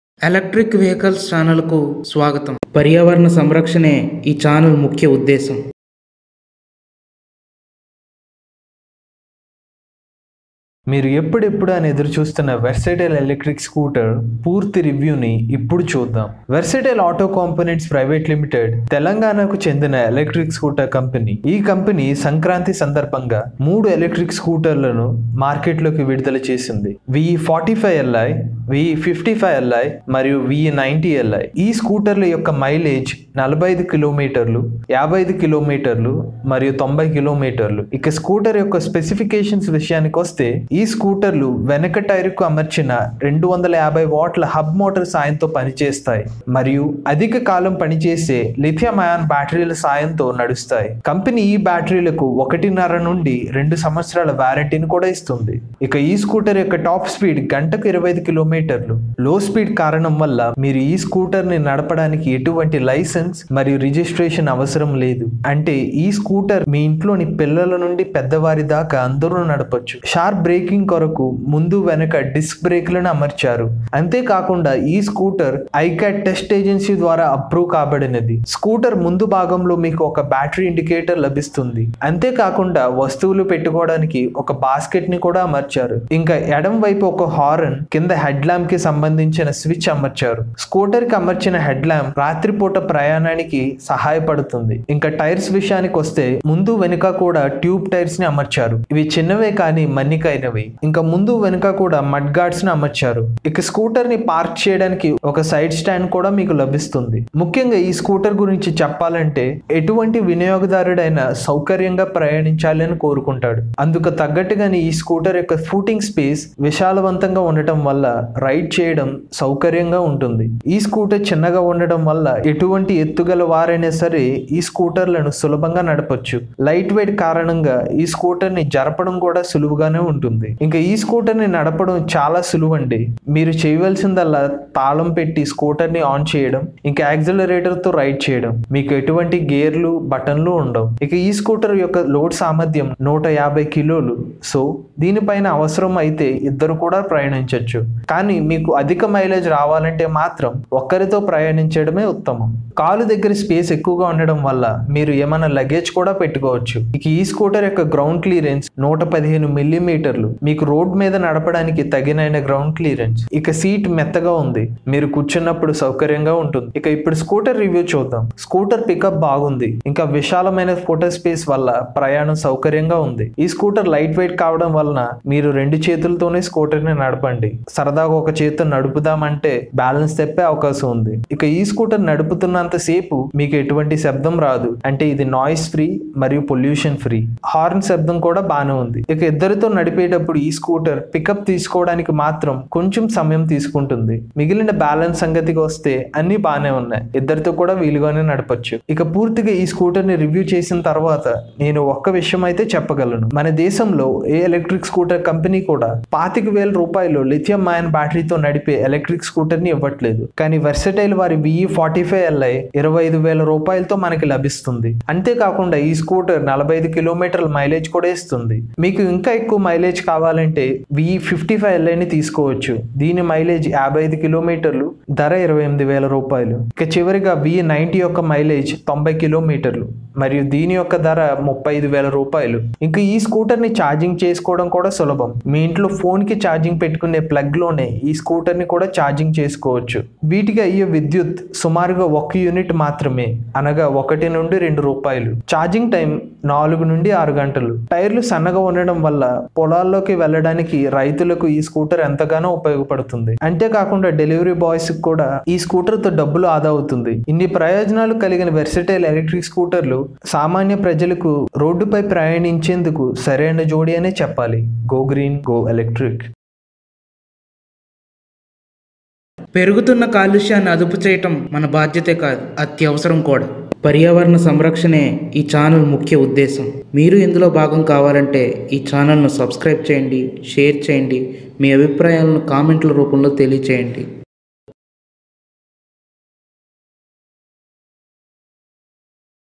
Verstaile VE 45 & 90 Electric Scooter Full Review: EV Telugu
Verstaile VE 45 & 90 Electric Scooter Full Review and Price details in Telugu. VE 45 is a made in India electric scooter which doesn't require license and registration to ride.

The electric scooter is manufactured in Hyderabad with a starting price of 25,000 rupees.